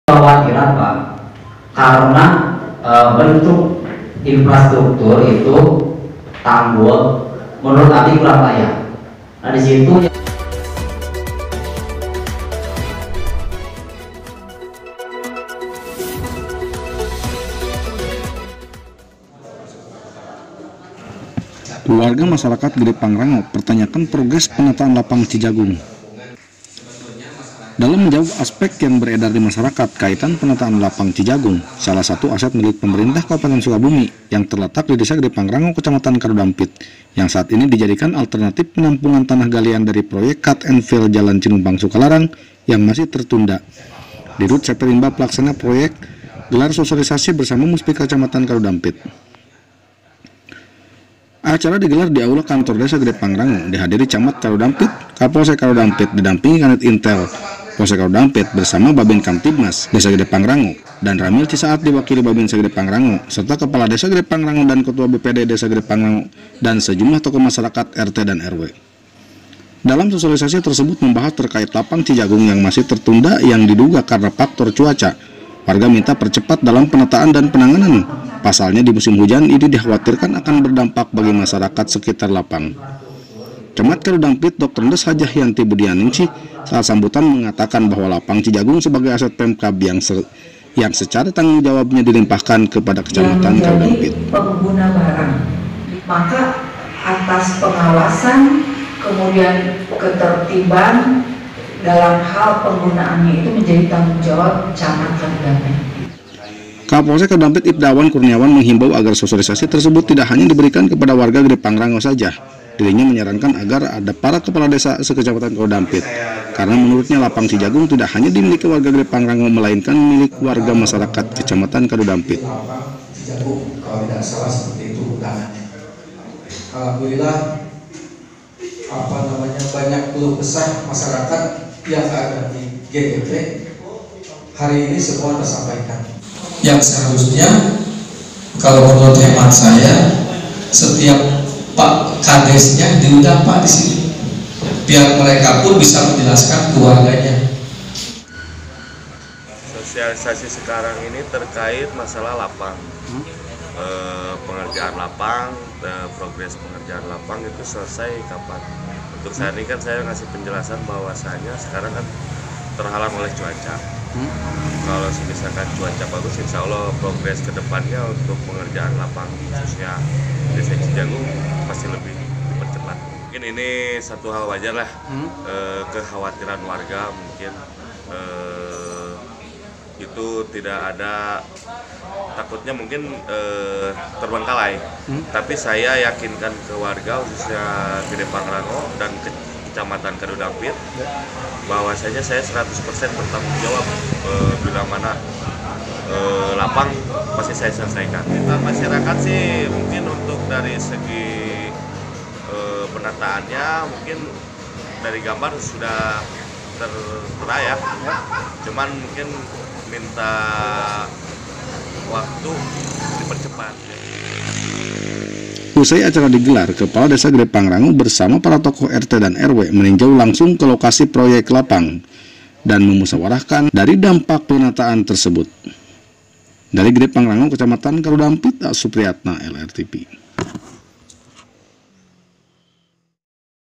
SOSIALISASI PENATAAN LAPANG CIJAGUNG, WARGA TANYAKAN PROGRES
Warga masyarakat gede pangrango pertanyakan Progres penataan Lapang Cijagung,

Lensareportase.com-Sukabumi.        Menjawab aspek yang beredar dimasyarakat kaitan penataan lapang Cijagung salah satu aset milik Pemerintah Kabupaten Sukabumi, yang terletak di Desa Gede pangrango Kecamatan Kadudampit yang saat ini di jadikan alternatif penampungan tanah galian dari peroyek cut and fill Jalan Cinumpang-Sukalarang yang masih tertunda, dirut CV Rimba pelaksana Proyek gelar Sosialisasi. Bersama muspika kecamatan kadudampit.

Acara di gelar di Aula Kantor desa Gede Pangrango Di hadiri Camat Kadudampit, Kapolsek Kadudampit di dampingi Kanit Intel Polsek Kadudampit, Bhabinkamtibmas Desa Gede Pangrango, Danramil Cisaat diwakili Bhabinsa Gede pangrango, Kepala Desa Gede pangrango dan Ketua BPD Desa Gede Pangrango serta sejumlah tokoh masyarakat RT dan RW. Senin (12/12/22).

Dalam sosialisasi tersebut membahas terkait Lapang Cijagung yang masih tertunda yang diduga karna faktor cuaca, warga minta percepat dalam penataan dan penanganan pasalnya di musim hujan ini dikhawatirkan akan berdampak bagi masyarakat sekitar lapang.

Camat kadudampit. Dra Hj. Yanti budiningsih  saat sambutan mengatakan bahwa lapang cijagung sebagai aset pemkab yang secara tanggung jawab nya di limpahkan ke Kecamatan Kadudampit.

"Saya camat kadudampit sebagai pengguna barang atas pengawasan, ketertiban dalam hal penggunaannya ( lapang Cijagung. red.) menjadi tanggung jawab Camat kadudampit, karna sampai saat ini lapang Cijagung ini masih tercatat sebagi aset milik pemda, Ketika ada tuntutan terhadap lapang Cijagung tuntutannya itu di tujukan kepada camat kadudampit, siapapun camatnya." Tandas Camat Kadudampit

Kapolsek Kadudampit IPDA Awan Kurniawan  menghimbau agar sosialisasi tersebut tidak hanya di berikan kepada warga Gede Pangrango saja, dirinya menyarankan agar ada para kepala Desa Sekecamatan kadudampit, karna menurutnya lapang Cijagung tidak hanya milik warga Gede pangrango melainkan milik Warga kecamatan kadudampit.

"Saya hanya menambahkan saja, kalo bisa sosialisasi ini agar diundang para kepala Desa Sekecamatan Kadudampit agar bisa menyampaikan kepada warganya, karna lapang Cijagung ini bukan saja milik warga Gede pangrango melainkan milik warga kecamatan kadudampit," Jelasnya.

Direktur pengembang CV Rimba Jaya Abadi Reza Tauhari selaku pelaksana Proyek saat di tanya wartawan usai kegiatan dirinya mejelaskan bahwa pihaknya bertanggung jawab 100% dalam penyelesaian penataan lapang kembali.

"Sosialisasi saat ini terkait masalah lapang, penataan lapang dan progres pengerjaan sampai kapan, nah ini saya kasih penjelasan sekarang, karna terhalang cuaca, kalo cuaca bagus, pasti lebih dipercepat untuk pengerjaannya", ujar Reza

"Mungkin ini satu hal yang wajar, kekhawatiran warga, takutnya mungkin terbengkalai, tapi saya yakinkan ke warga, khususnya warga gede pangrango kecamatan kadudampit bahwa saya 100 % akan bertanggung jawab bila lapang Cijagung akan di selesaikan." Jelasnya.

Usai acara di gelar kepala desa Gede pangrango bersama para tokoh RT dan Rw meninjau langsung kolokasi proyek lapang cijagung, dan memusyawarahkan dampak dari penataan tersebut.

Rep. A. Supriatna
Smi.
Red.